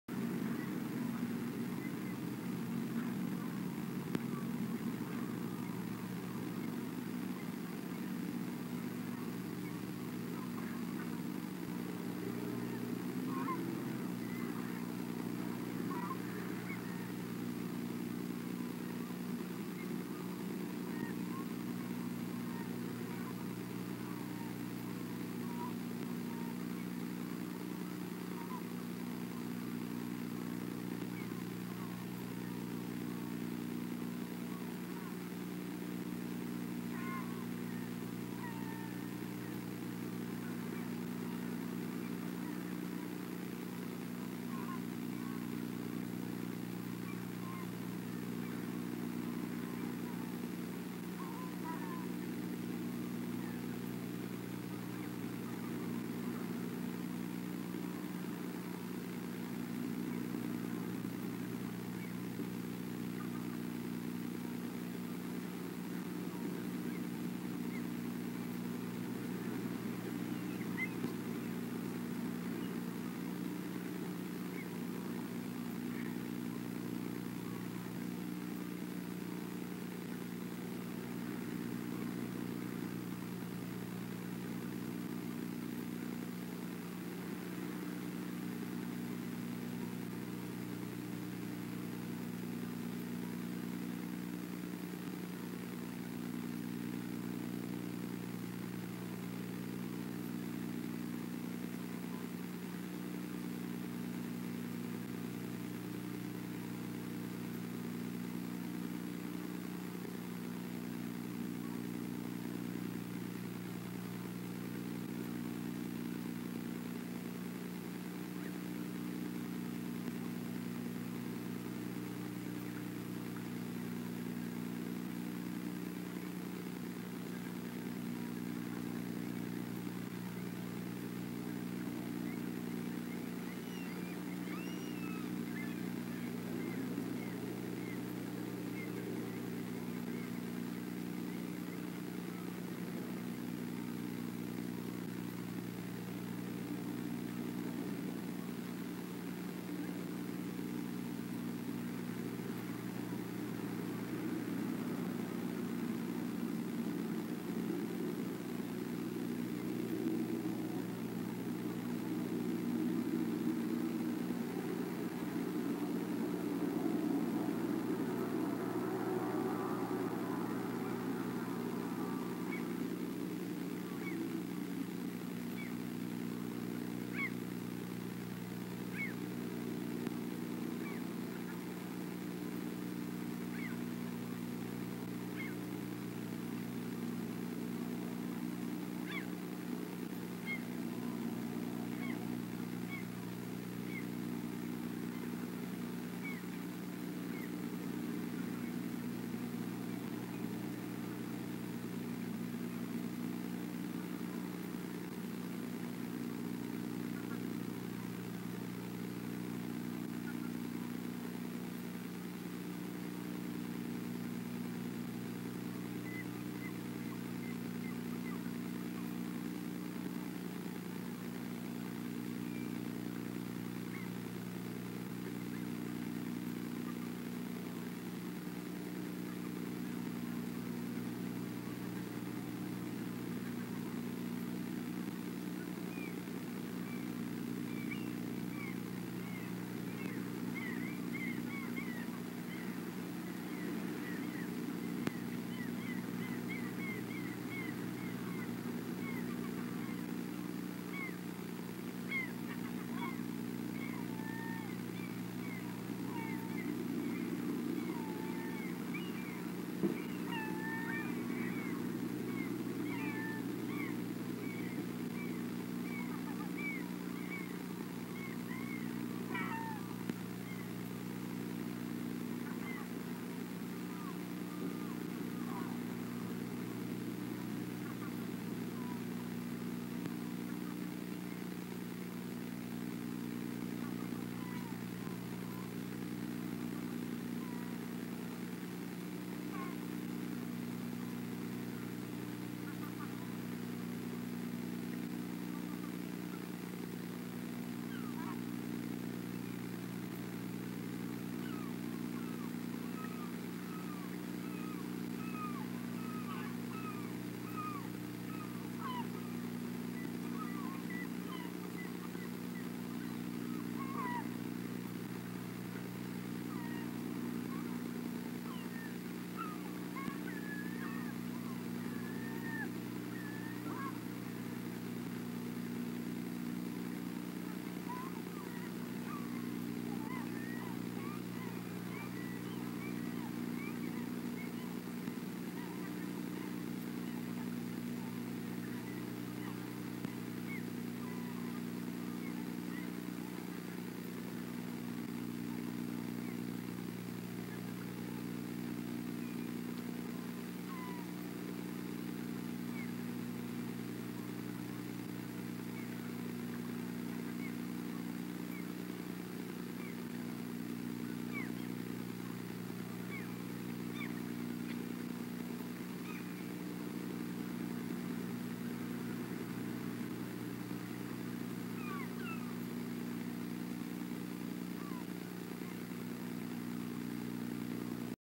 2018 04 18 /19 10 CET
Beluga whales in Naknek River , King Salmon Alaska
at 3:00 you can see the beluga with the dark fin